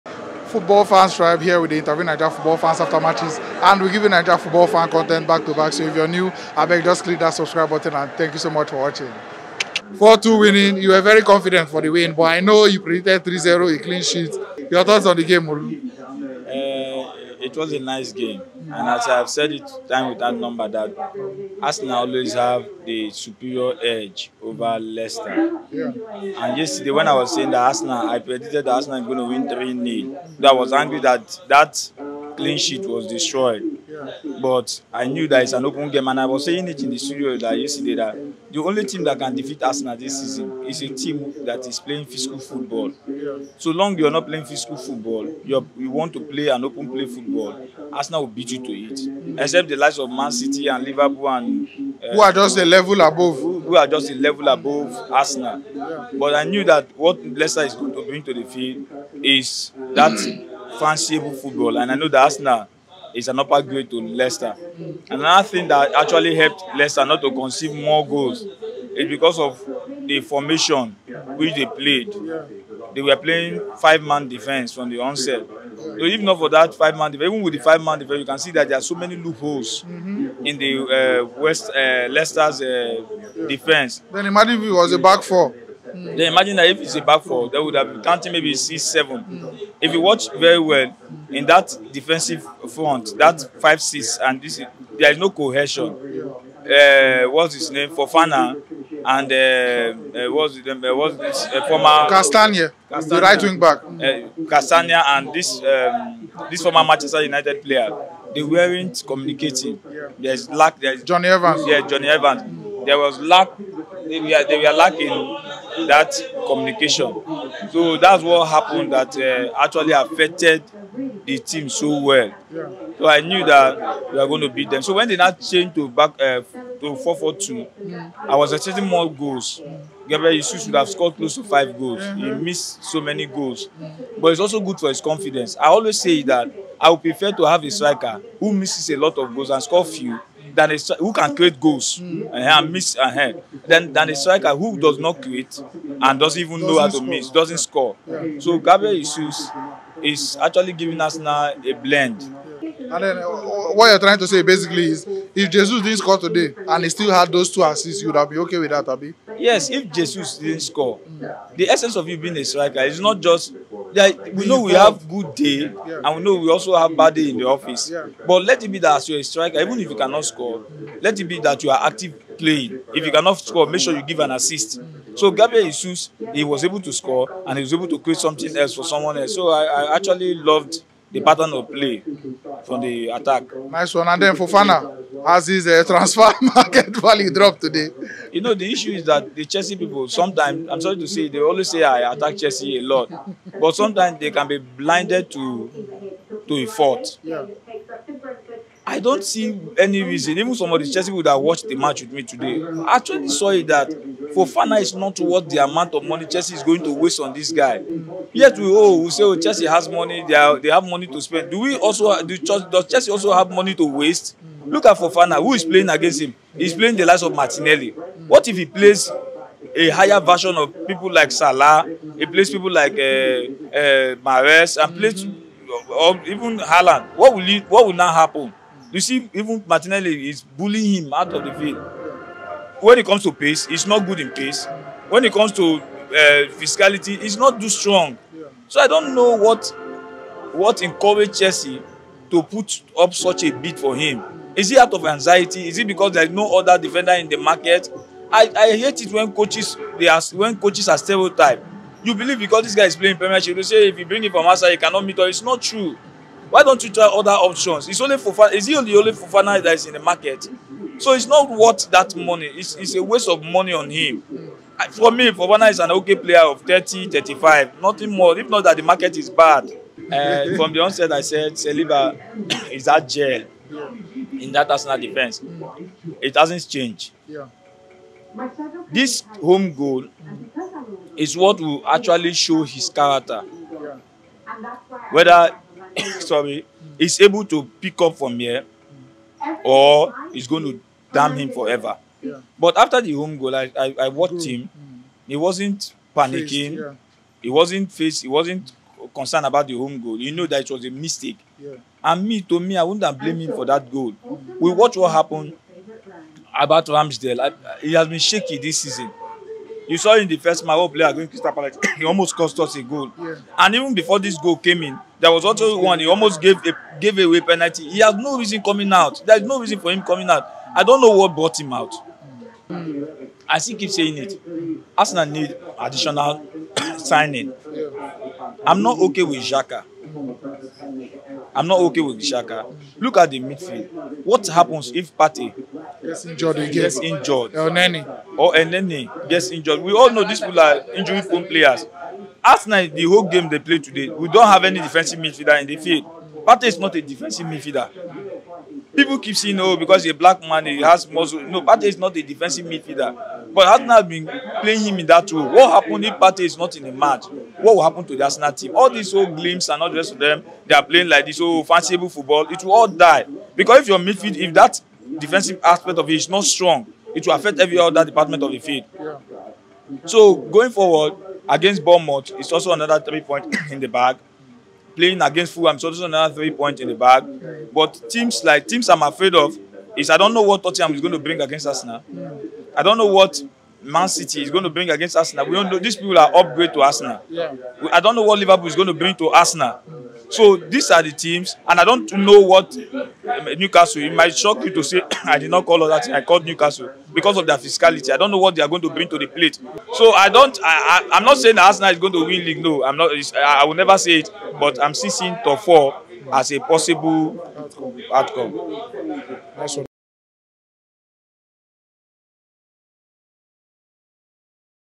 ARSENAL 4-2 LEICESTER ( Olu NIGERIAN FAN REACTION) - PREMIER LEAGUE HIGHLIGHTS 2022-23
Mactch reaction from a viewing center in Nigeria from Olu saying Arsenal have the superior power over Leicester city, also the only thing that helped them was the defensive formation today

Proposed next Game 👇🏽
EPL